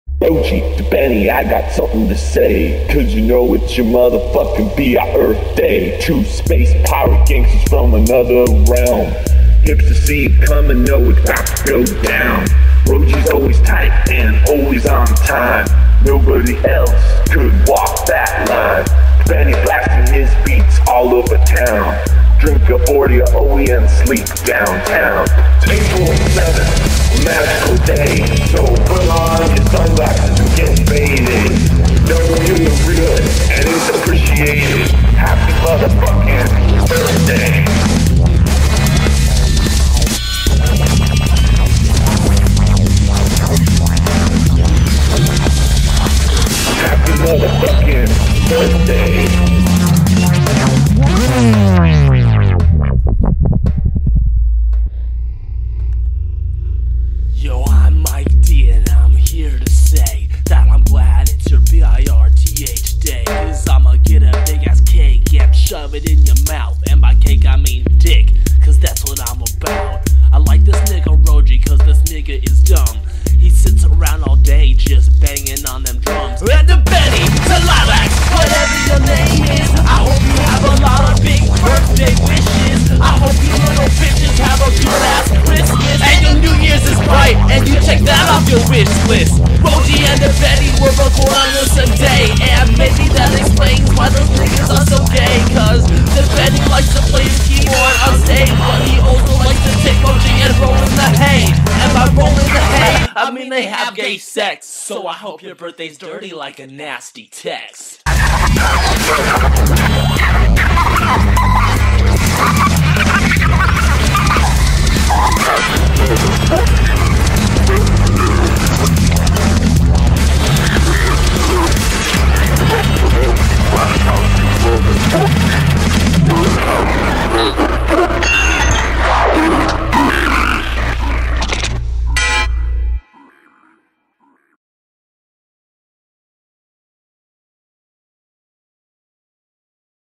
HAPPY MUTHAFUCKIN' BIRTHDAY to D. Bene & Ro-G 2010
in 2010, on this very day, Mike Diva and I kicked out these rhymes to celebrate the birthdays of two badass mofos.  Rogie Lucero and D. Bene Tleilax... NOW.  That time has come again.